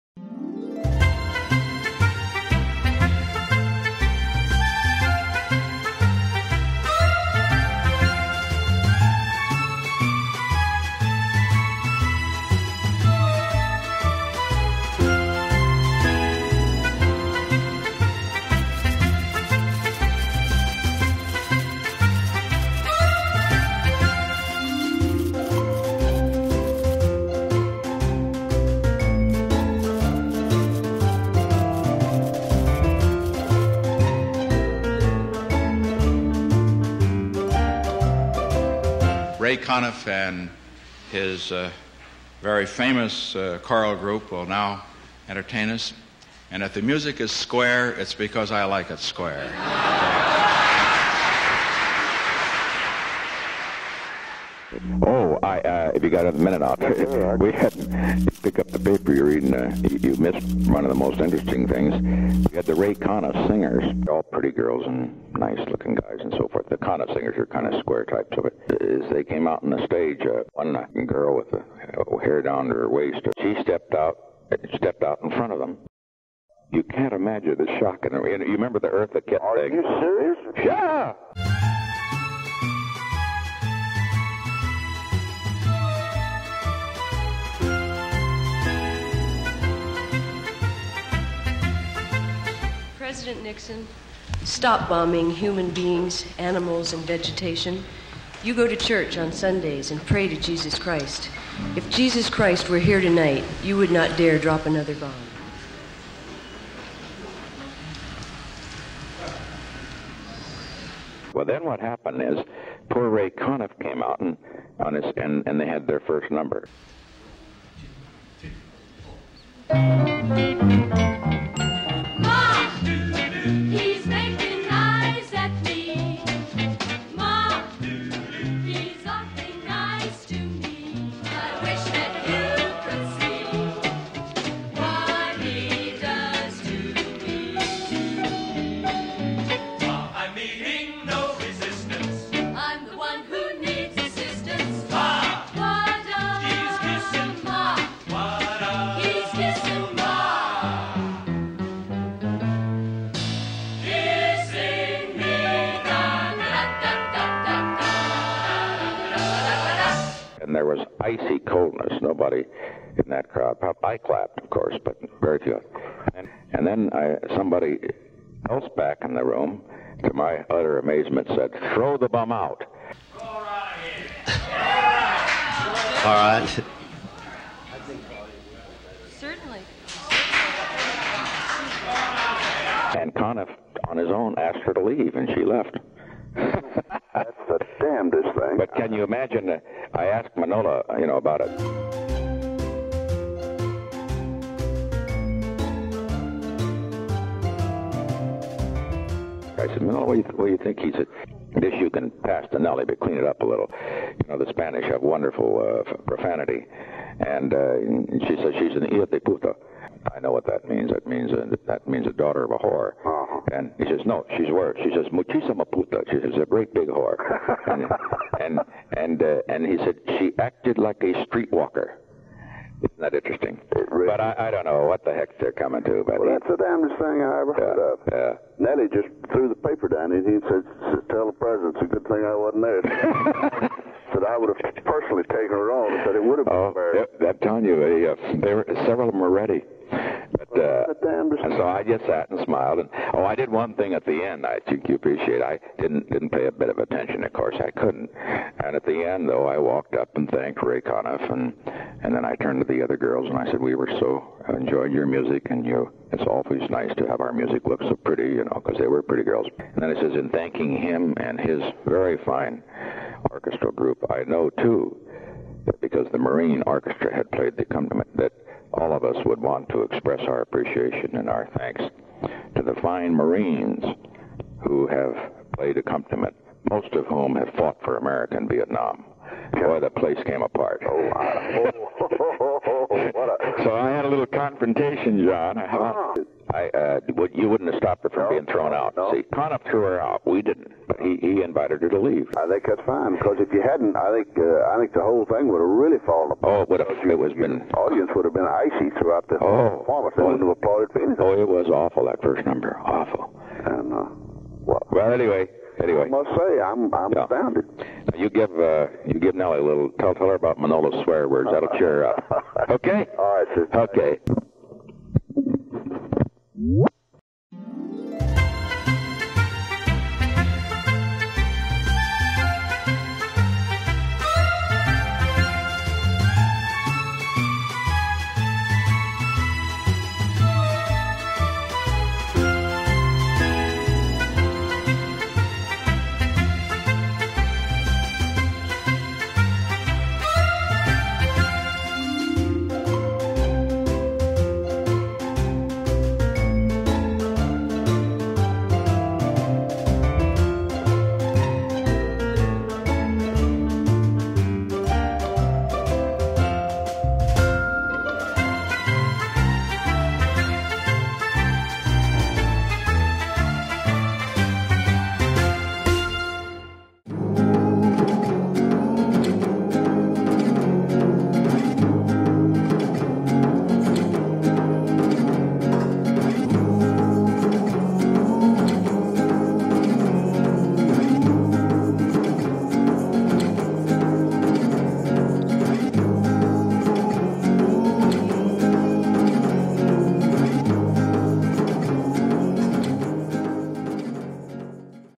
Rebel Without Applause: The Anti-War Protestor Who Left Nixon and the White House Speechless
In 1972, Carole Feraci concocted a plan to protest Richard Nixon while performing at a white-tie event at the White House, during the height of public opposition to the war.